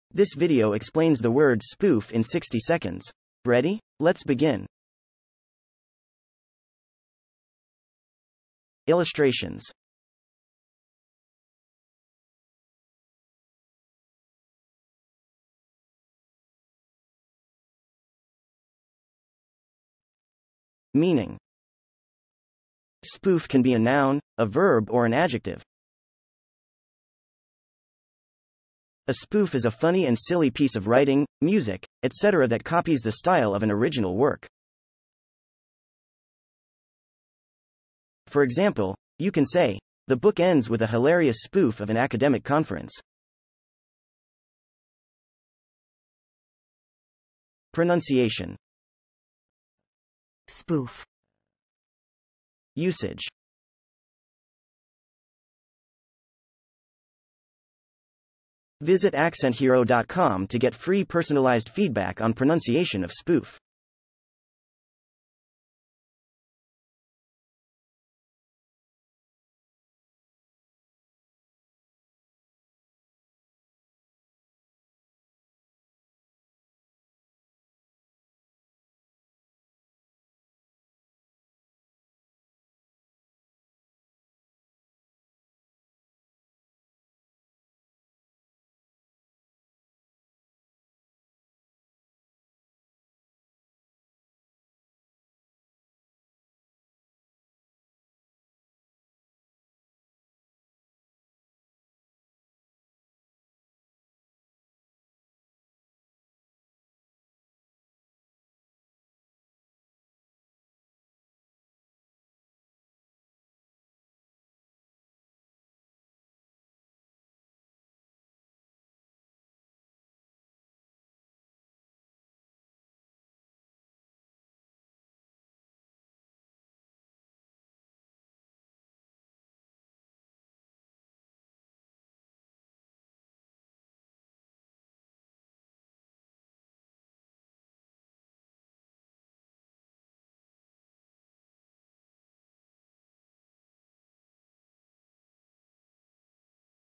SPOOF - Meaning and Pronunciation
What spoof means? How to pronounce spoof?
This video explains the meaning of the word spoof and provides examples of American English pronunciations by male and female speakers.

Chapters:
0:00 Intro
0:08 Illustrations
0:22 Meaning
0:48 Pronunciation
0:53 Native speaker use
NaN:NaN Service Promotion

Definition of spoof: A spoof is a funny and silly piece of writing, music, etc. that copies the style of an original work.

IPA Transcription of spoof: /spuf/.

If you'd like to test your pronunciation of spoof, check out Accent Hero's real-time pronunciation feedback tool: